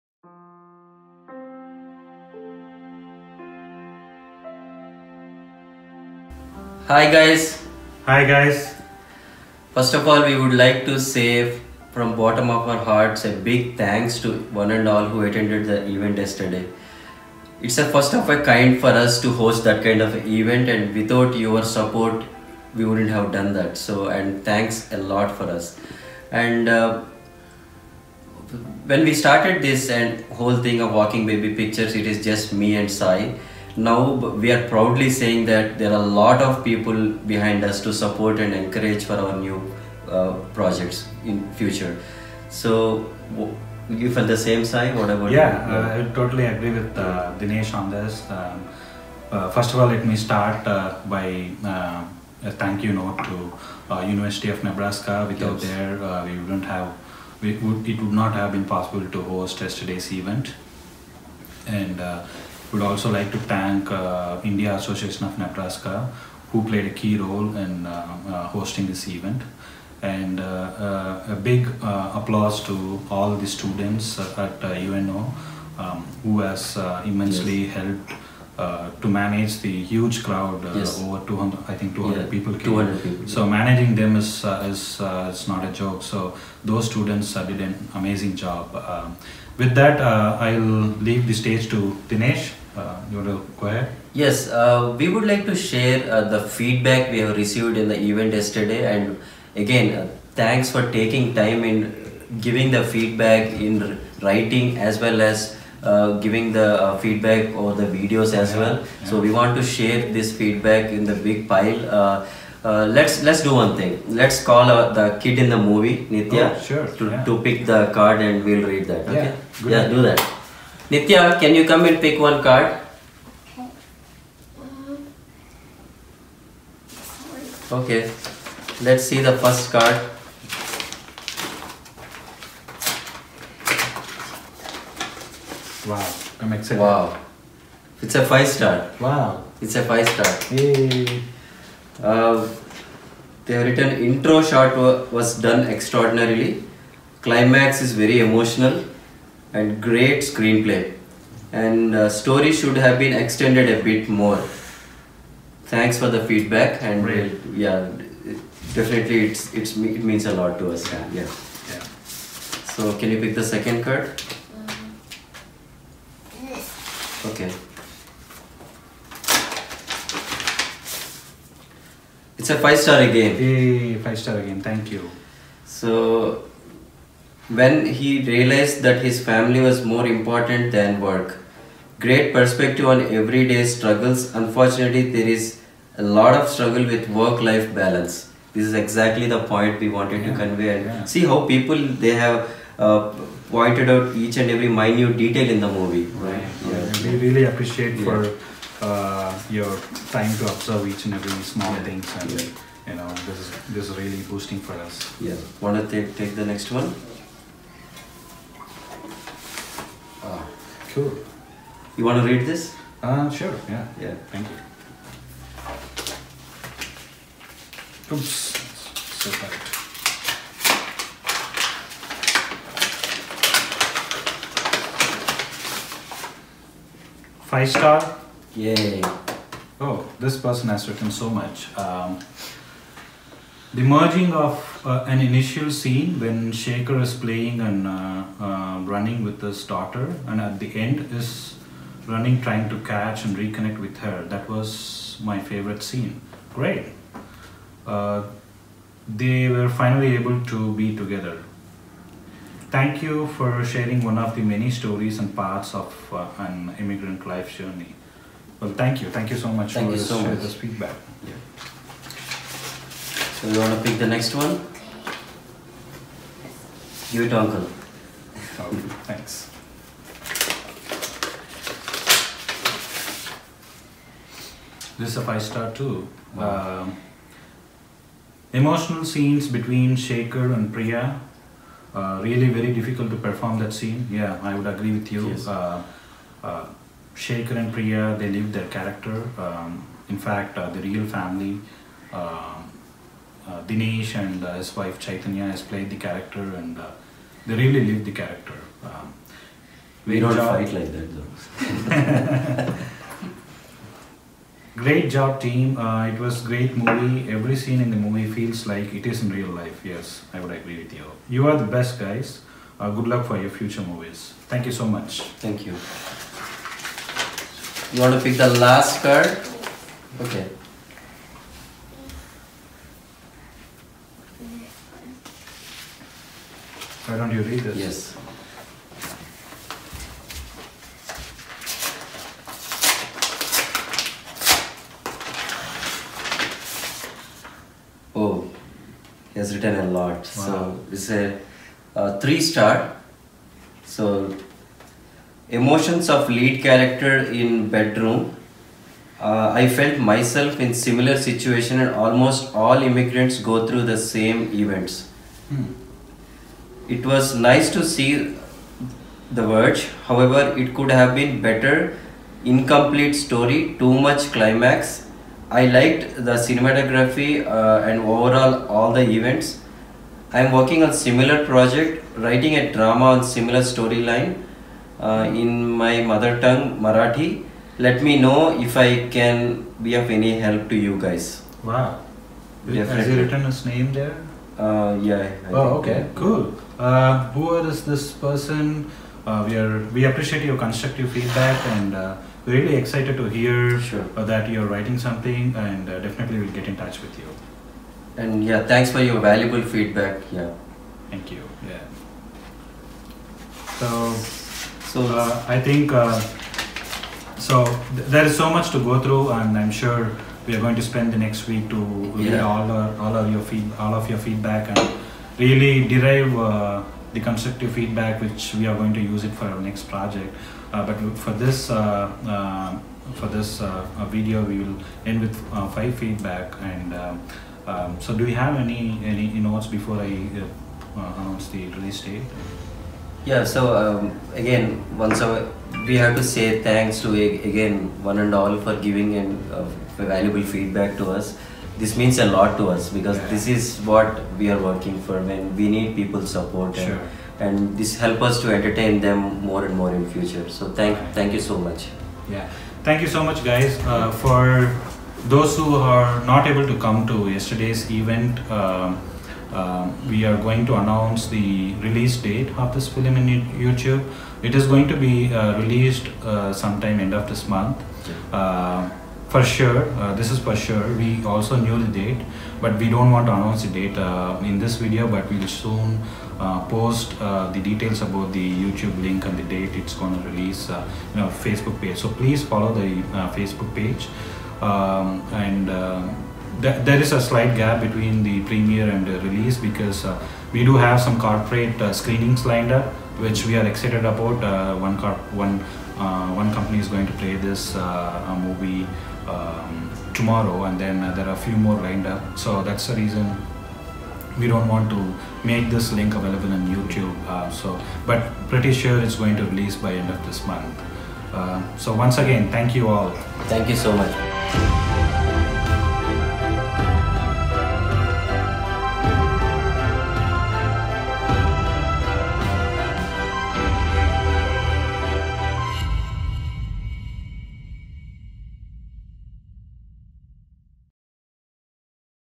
Verge Audience Feedback | Online release announcement
Audience feedback readout from the Verge premiere screening